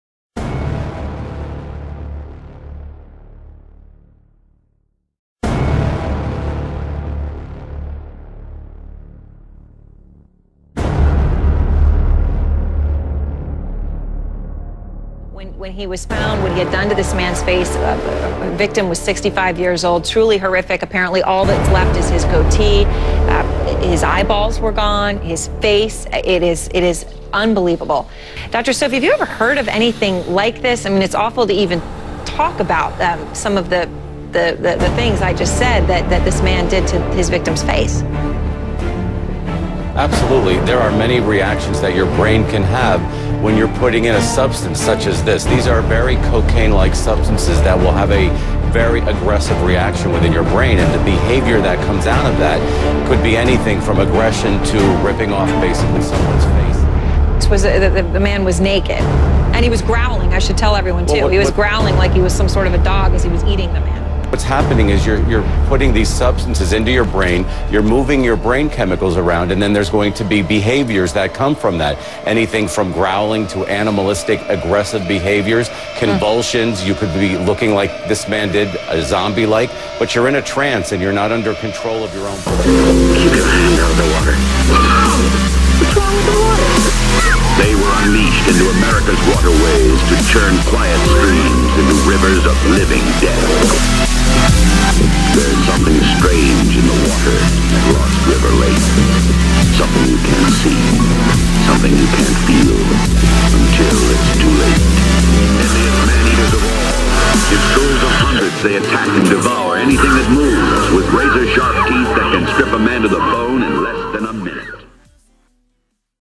No, Your Not Dreaming
Romans 8:5 - 8 "For they that are according to the flesh, mind the things that are of the flesh; but they that are according to the spirit, mind the things that are of the spirit. For the mind of the flesh is death; but the mind of the Spirit is life and peace: Because the mind of the flesh is enmity against God: for it is not subject to the law of God; for neither indeed can it be:That's why those who are still under the control of their own sinful nature can never be pleasing to God."

Flee from sin and be sober minded, pray to God through his son Jesus Christ who died on the cross and was raised from the dead and you will live. Pray to God also that you may escape what has and is coming upon the world!

Zombie Nation : Bath Salts said to make a Person act like a Wild Animal (May 30, 2012)